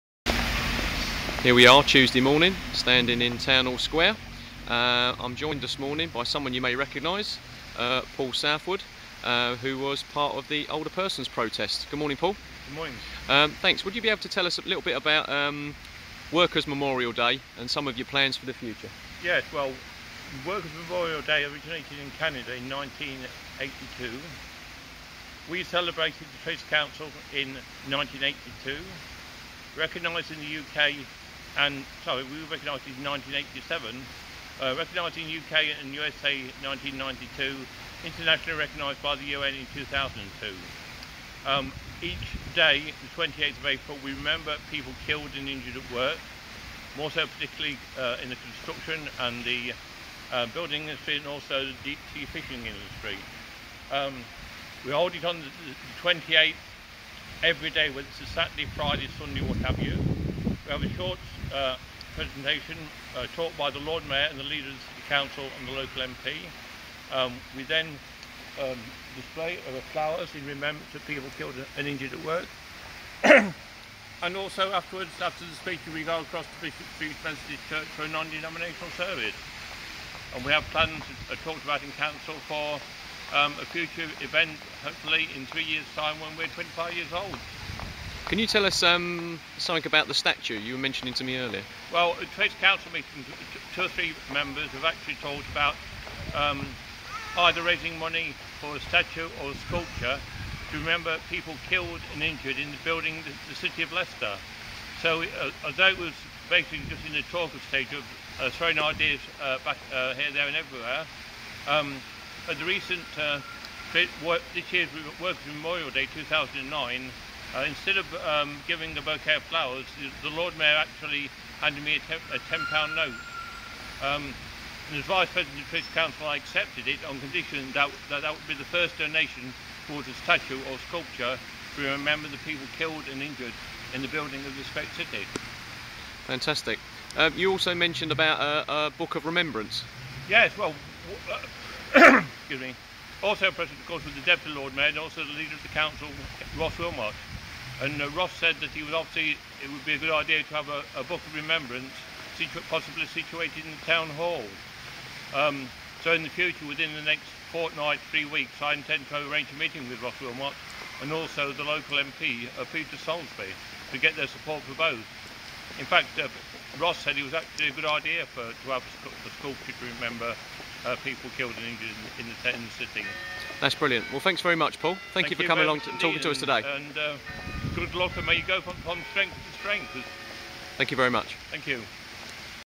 Paul Southwood & Workers' Memorial Day
Paul Southwood talking about Workers' Memorial Day in Leicester.